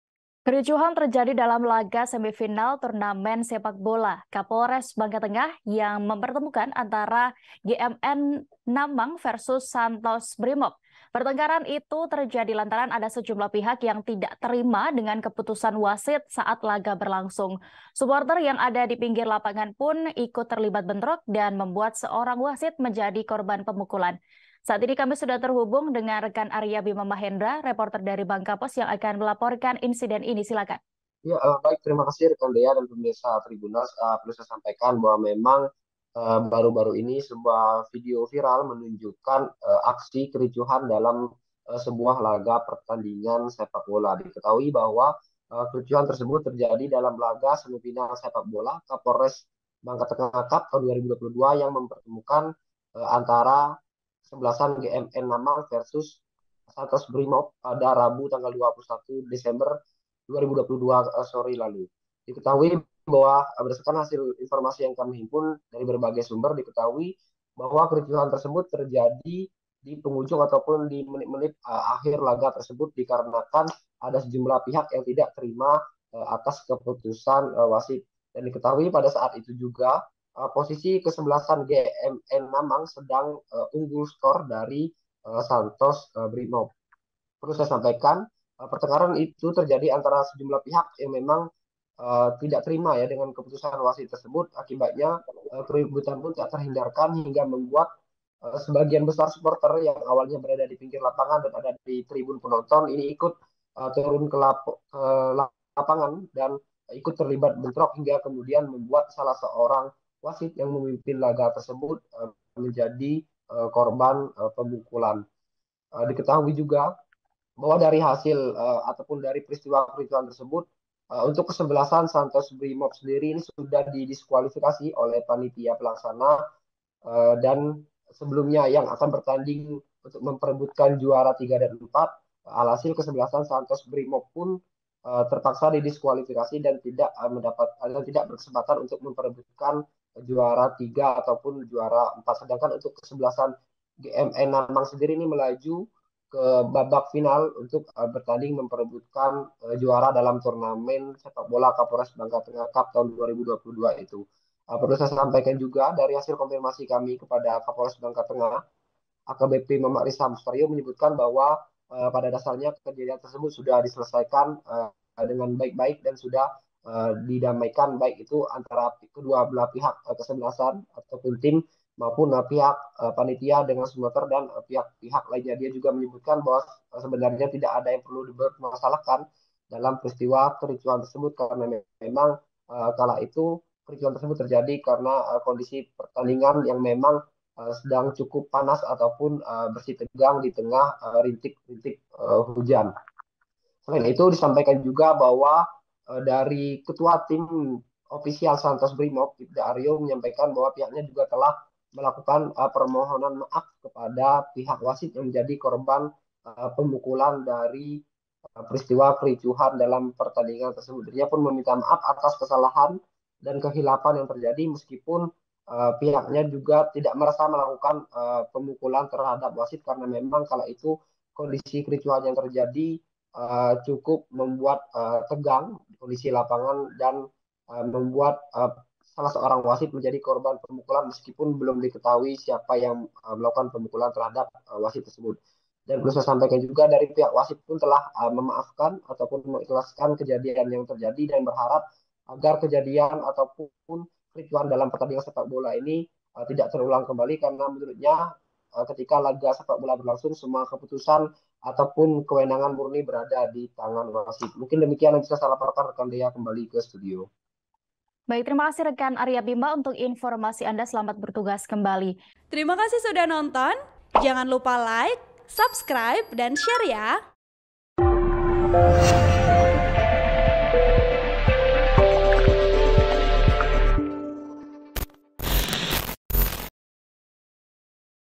Bentrokan saat Turnamen Kapolres Bangka Tengah Cup GMN Namang Vs Santos Brimob, Wasit Jadi Korban
TRIBUN-VIDEO.COM - Kericuhan terjadi dalam laga semifinal turnamen sepakbola Kapolres Bangka Tengah yang mempertemukan antara GMN Namang vs Santos Brimob.

Pertengkaran itu terjadi lantaran ada sejumlah pihak yang tidak terima dengan keputusan wasit saat laga berlangsung.

Suporter yang ada dipinggir lapangan pun ikut terlibat bentrok dan membuat seorang wasit menjadi korban pemukulan.

Kapolres Bangka Tengah, AKBP Moch. Risya Mustario menyebutkan bahwa kejadian itu sudah diselesaikan dengan baik-baik.

Santos Brimob juga langsung menyampaikan permintaan maaf atas insiden yang terjadi.(*)